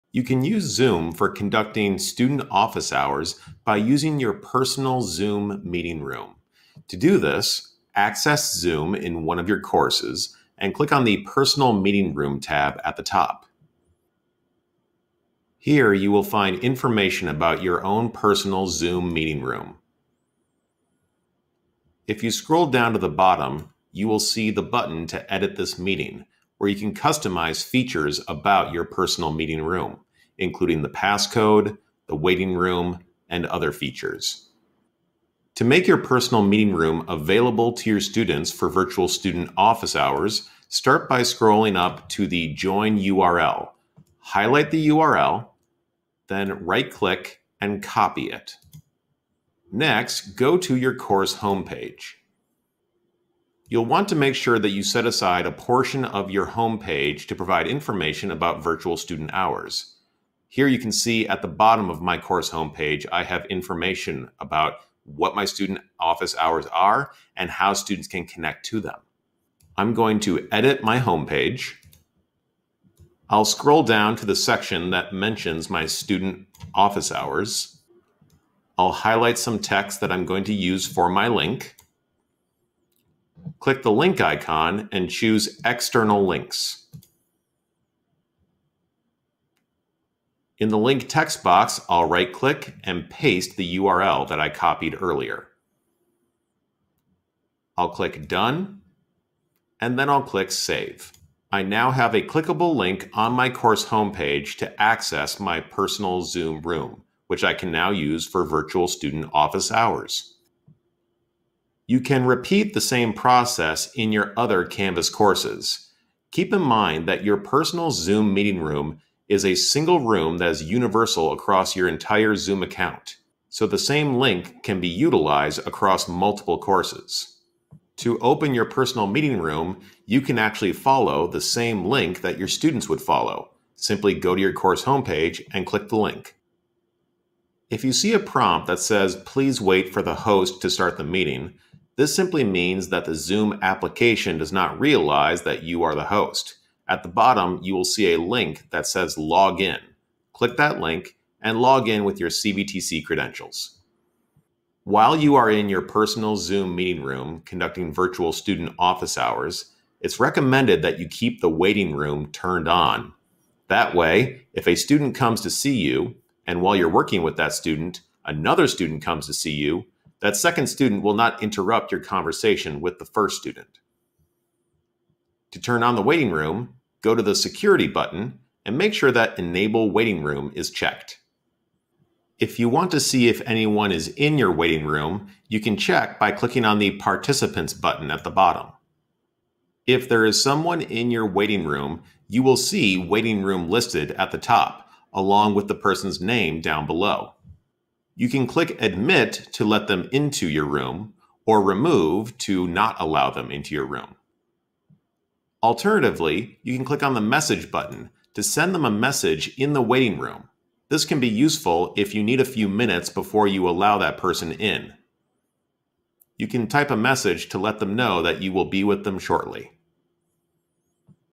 Using Zoom for Virtual Student Hours
A quick overview of how to use your personal Zoom meeting room to conduct virtual student office hours